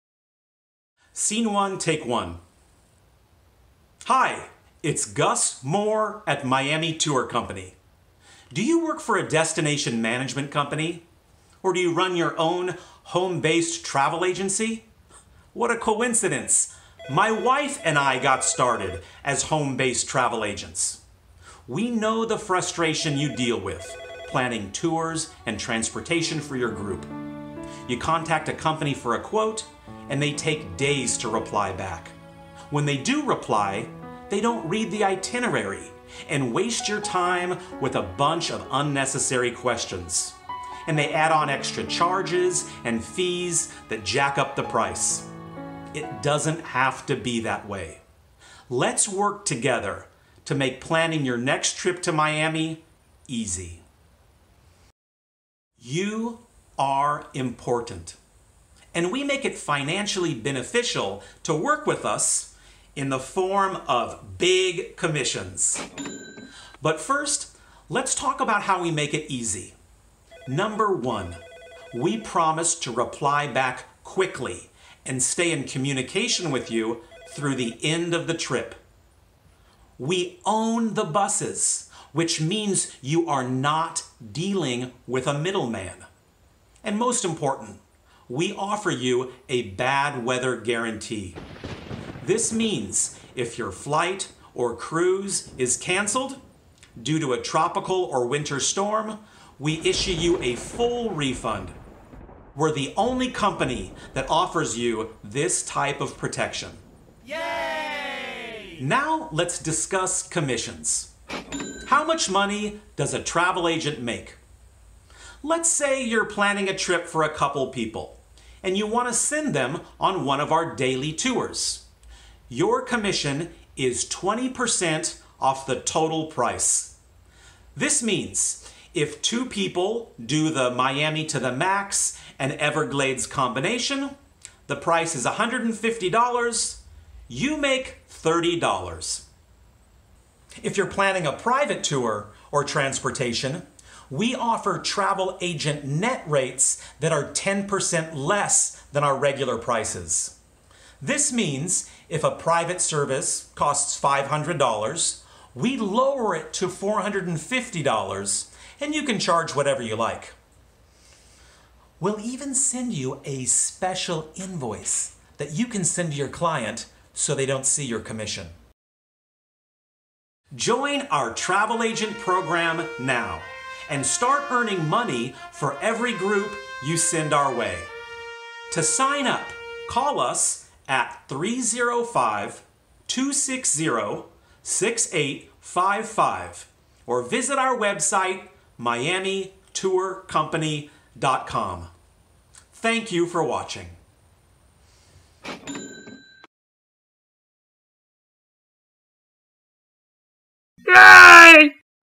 How Travel Agents Work With Us
Do you work for a DMC, or run you run your own travel agency? Miami Tour Company got our start as home-based travel agents?